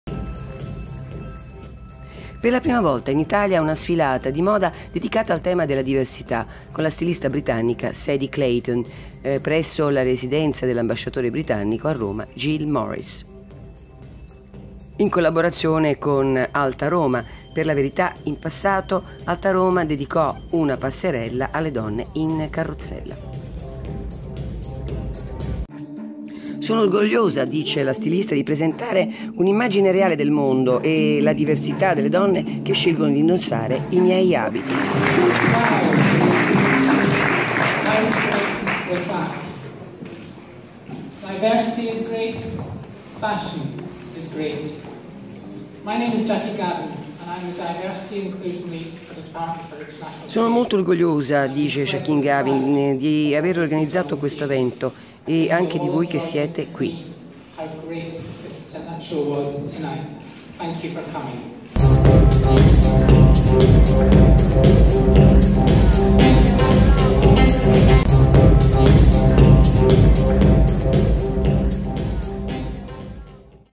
'Fashion is great with diversity
Una paaserella alla residenza britannica dedicata alla diversita.la moda come messaggio sociale